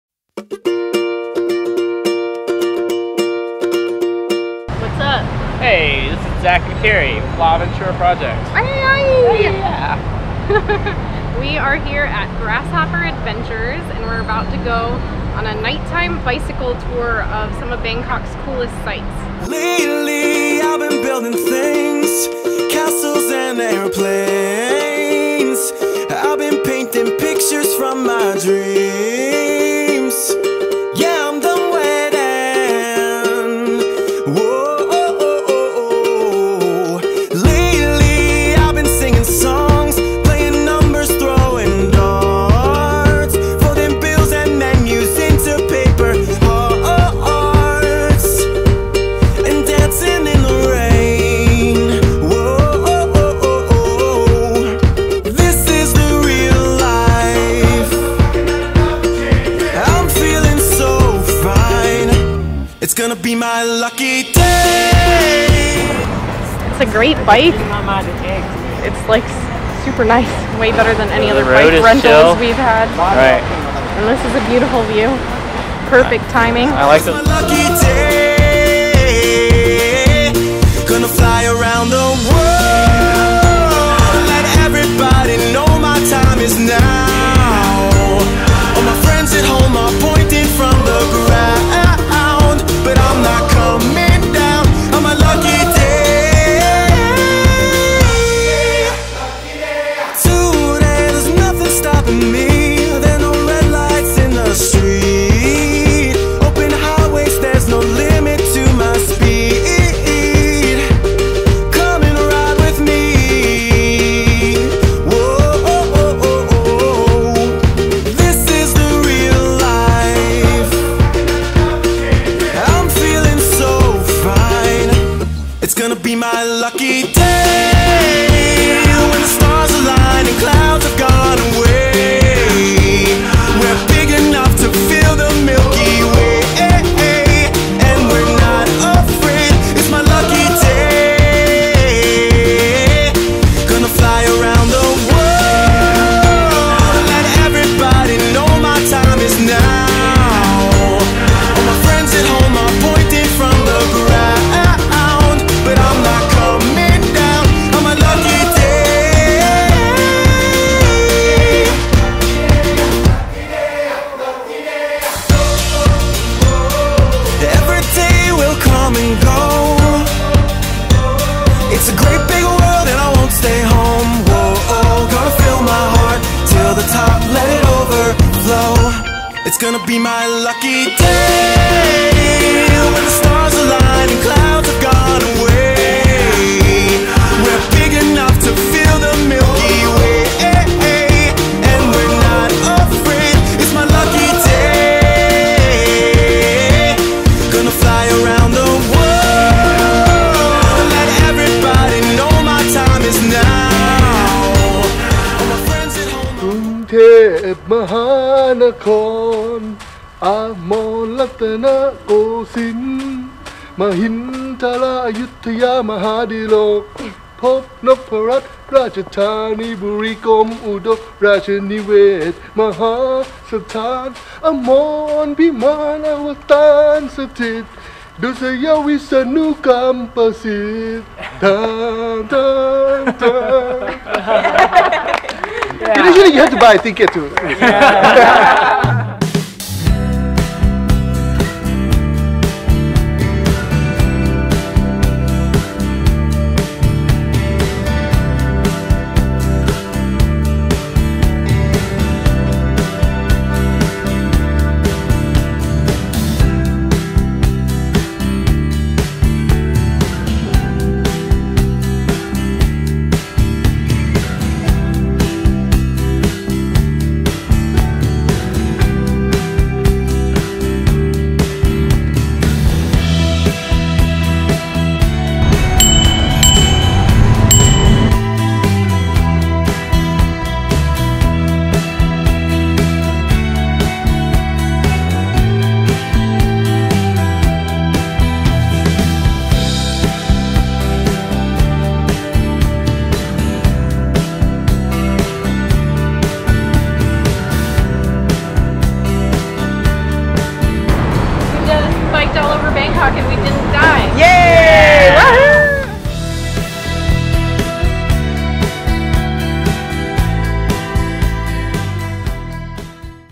Grasshopper Night Bike
Grasshopper Adventures runs bicycle tours all over Asia, with expert local guides who show you an amazing time!  The Bangkok Night Bike tour takes you to several of the coolest temples in Thailand (minus the daytime crowds), across the Chao Phraya River, and to the famous Flower Market!  Plus you get lots of good street snacks included in the price!  The guides are very knowledgeable and help you stay safe in the crazy traffic!